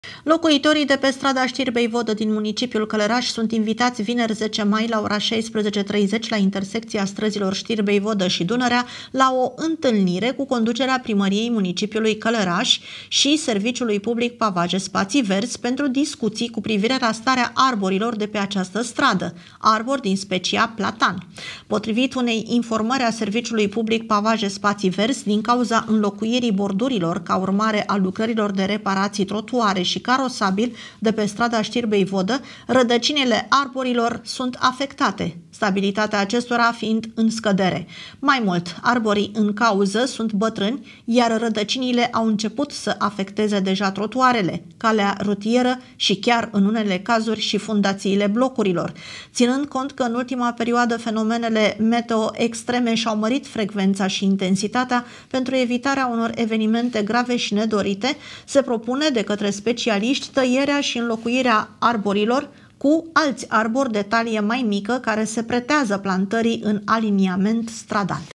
Știri Locale - Față în față cu reprezentanții Primăriei Călărași
Știri Locale

Vineri, 10 mai,  de la ora 16:30, conducerea Primăriei Mun. Călărași, reprezentanți ai Serviciului Public Pavaje Spații Verzi si locuitorii vor purta discuții cu privire la starea arborilor de pe strada Știrbei Vodă.